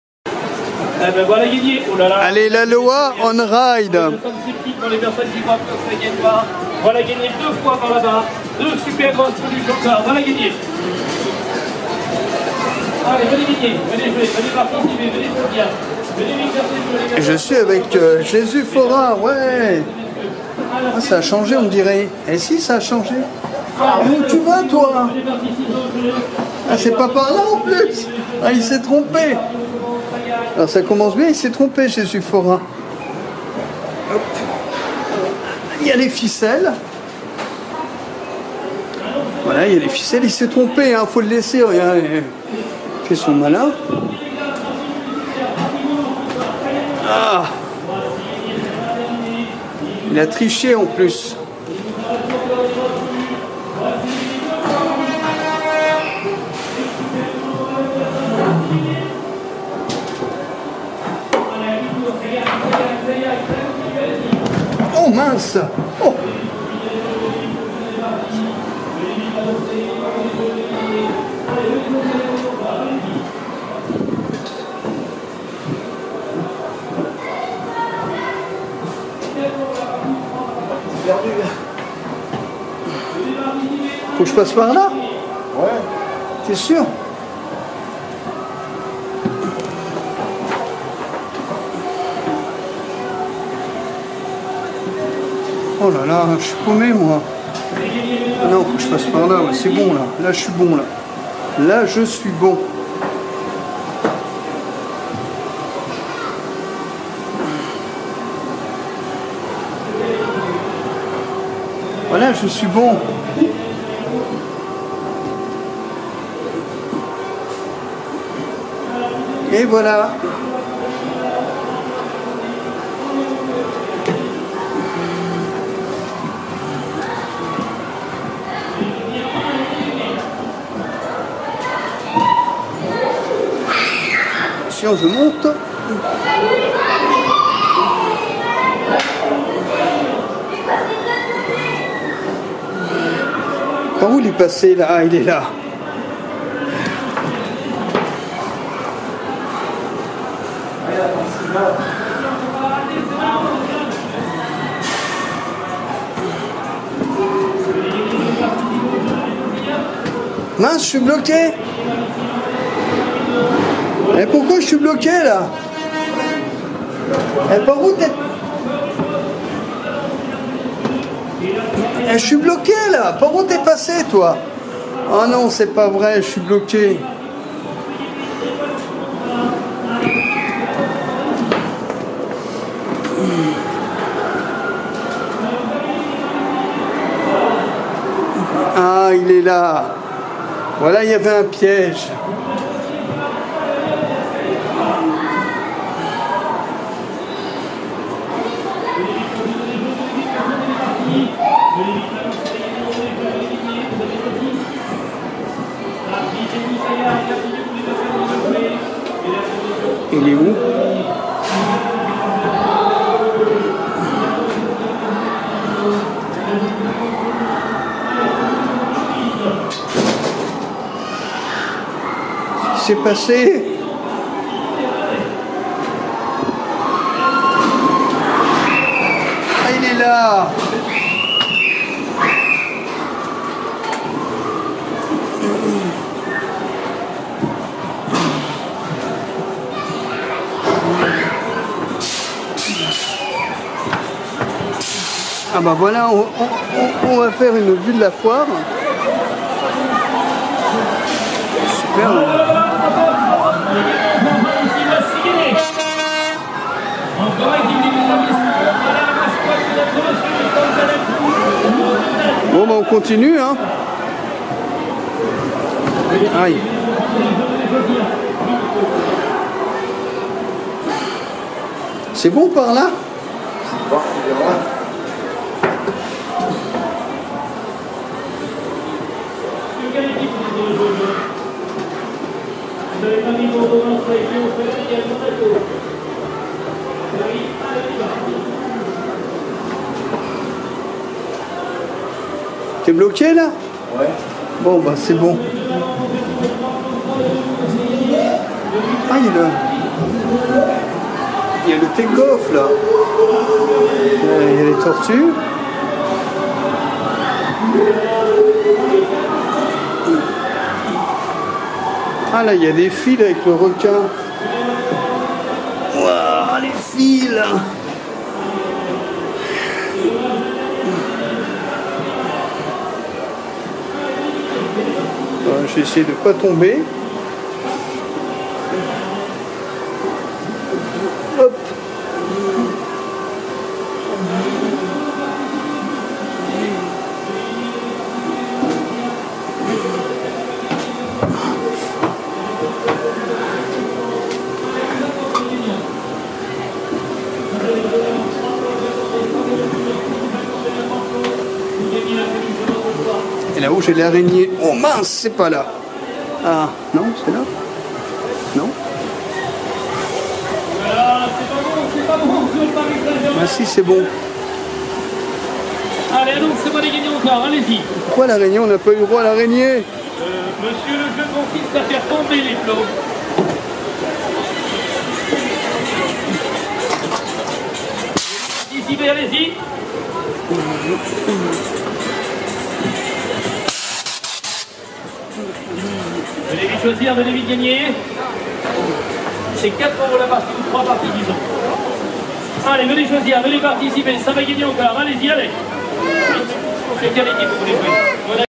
Aloha onride - Bordeaux octobre 2016
Le labyrinthe Aloha on ride avec Jesus Forain du site Turbo Kermis, à la foire aux plaisirs de Bordeaux le samedi 8 octobre 2016.